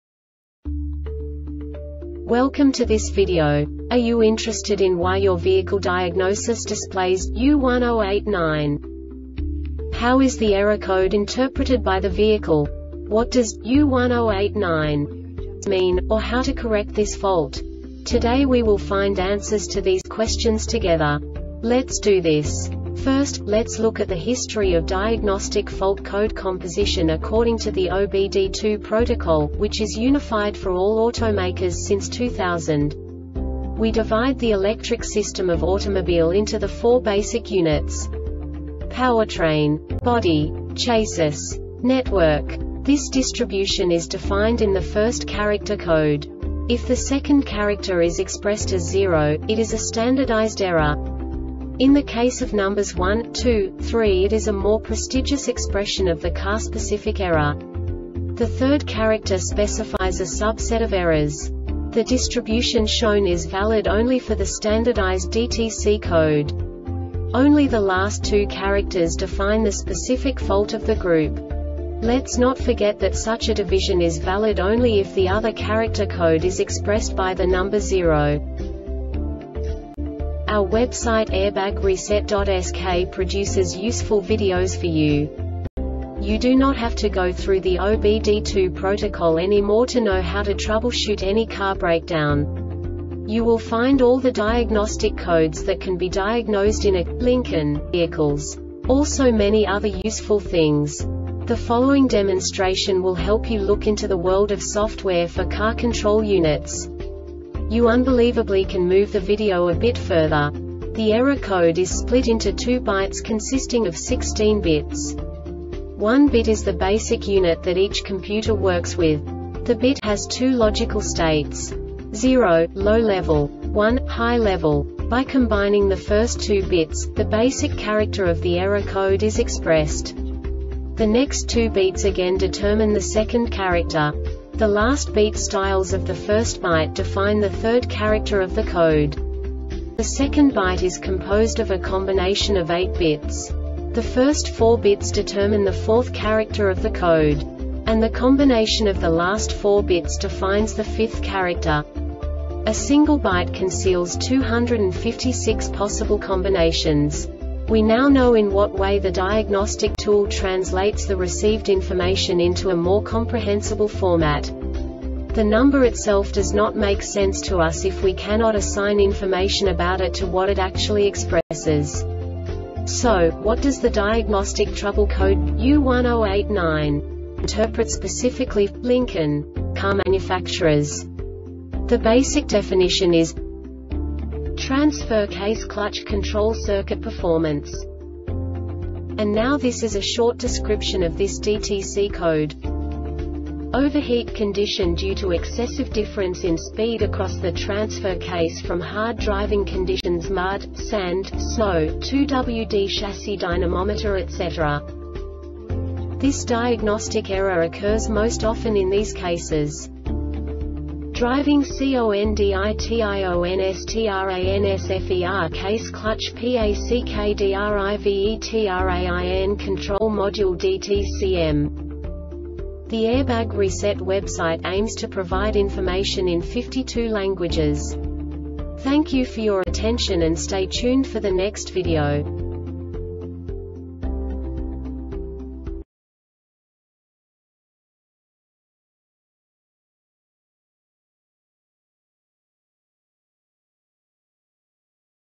DTC Lincoln U1089 Short Explanation
Contents:
0:21 Basic DTC analysis according to OBD2 protocol standard.
1:48 Insight into programming
2:58 A diagnostically understandable description of the Diagnostic Trouble Code
3:05 Basic error definition

Make:
Lincoln
Code:
U1089
Definition:
Transfer Case Clutch Control Circuit Performance
Description:
Overheat condition due to excessive difference in speed across the Transfer Case from hard driving conditions (Mud, Sand, Snow, 2WD Chassis dynamometer etc.).
Cause:
DRIVING CONDITIONSTRANSFER CASE CLUTCH PACKDRIVETRAIN CONTROL MODULE (DTCM)